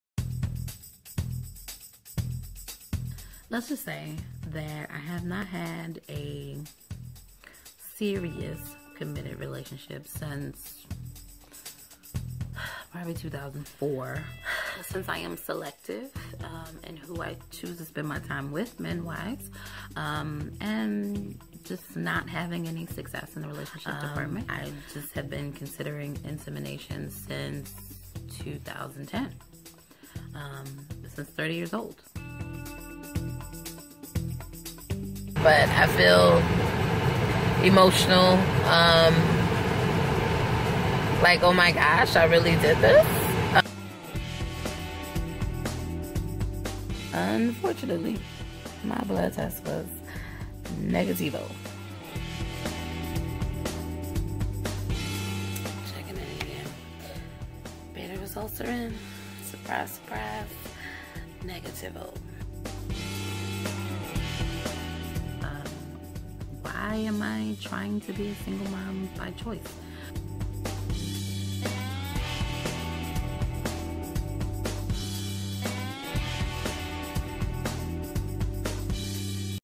Tamikia TTC: Welcome to My Channel!
I'm on a journey to become a Single Mother By Choice (SMBC).  At age 37 with no significant other, becoming a mom is way more important than becoming a wife.  While I am still open to meeting and building a life with someone, my priorities have shifted.  Thank you for taking this journey with me.